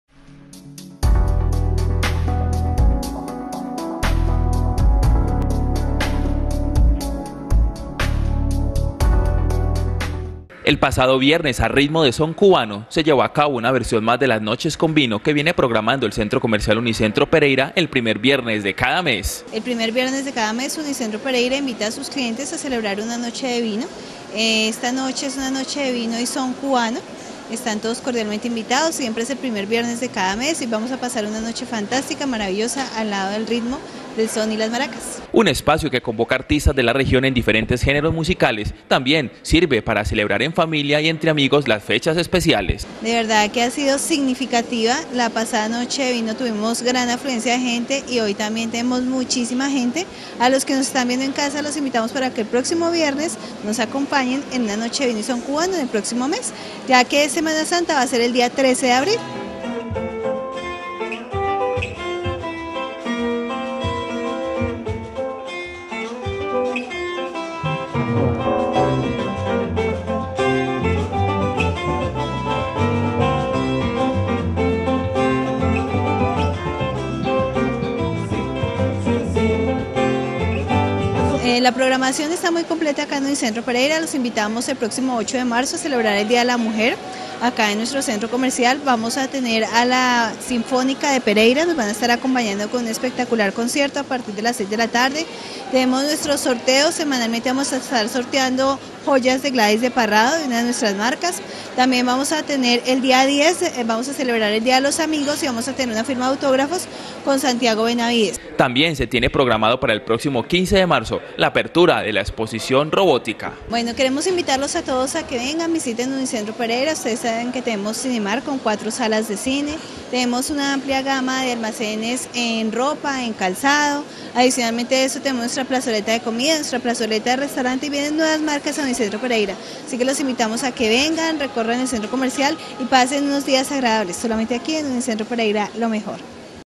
NOCHE DE VINOS EN UNICENTRO PEREIRA
Diferentes actividades culturales y el habitual espacio de la noche del vino, son algunas de las sorpresas que el centro comercial unicentro Pereira programó para sus visitantes en la   fecha  del  día de la mujer.

Periodista: Alejandro Patiño
Camarógrafo: Edgar Linares
Edición: Mauricio Díaz
Editor informativo: Gustavo Colorado